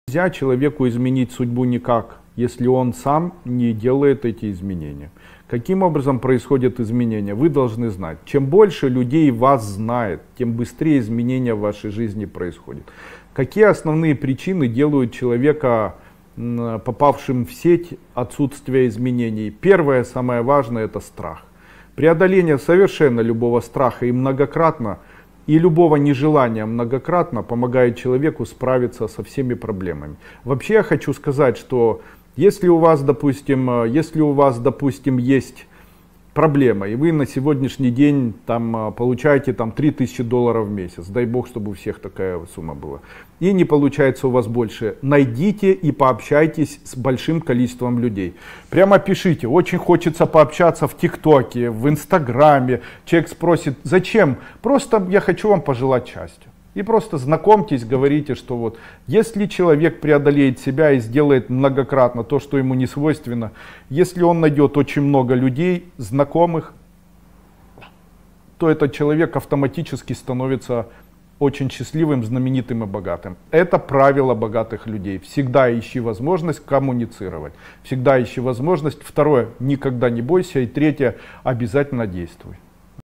Как преодолеть страхи и нежелания для достижения счастья
Этот день настал: Узнайте главный секрет богатых от Эзотерического Гуру!
В этом видео мы обсудим, как преодолеть страхи и нежелания, которые мешают вам стать богатым и счастливым. Вы узнаете о том, что изменения в жизни начинаются с вас, и как важно находить возможности для общения с другими людьми. Мы разберем основные причины, по которым люди остаются в сети отсутствия изменений, и как страх может стать вашим главным препятствием.
Научитесь действовать несмотря на свои страхи! Мы поделимся с вами практическими советами, как расширить свой круг общения и наладить важные связи. Узнайте, почему общение с большим количеством людей может радикально изменить вашу жизнь и помочь вам достичь желаемого успеха.
Не упустите возможность изменить свою судьбу и стать счастливым и богатым!